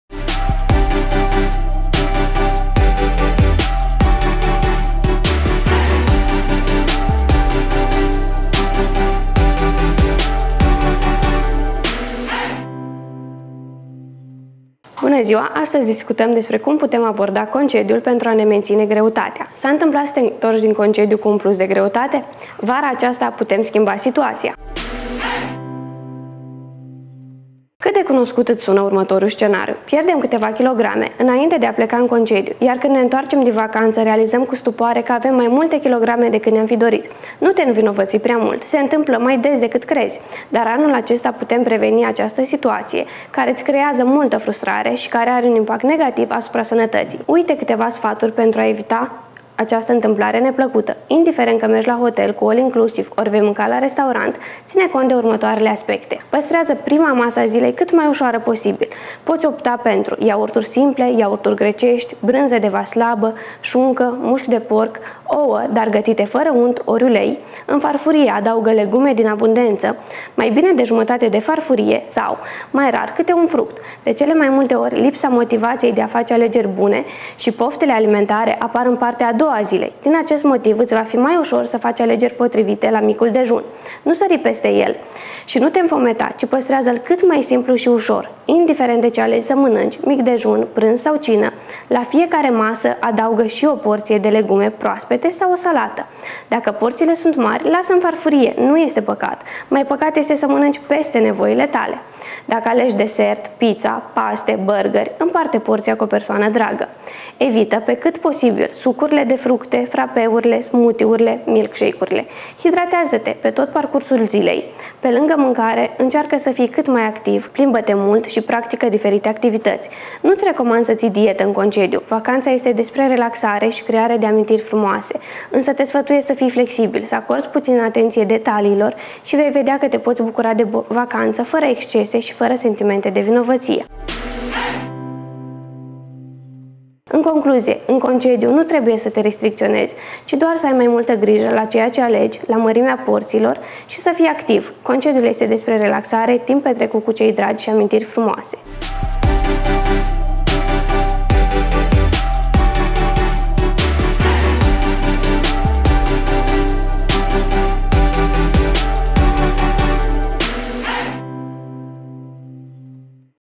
Sfatul specialistului: ce să mâncăm în concediu pentru a ne păstra silueta
Se poate să nu ne întoarcem din concediu cu câteva kilograme în plus? Răspuns pozitiv, dacă urmăm cu strictețe sfatul unui specialist. În concediu nu trebuie să te restricționezi, ci doar să ai mai multă grijă la ceea ce alegi, la mărimea porțiilor și să fii activ, dă asigurări Andra Spatar, nutriționist dietetician. Urmează rubrica ”Fii bine cu tine”!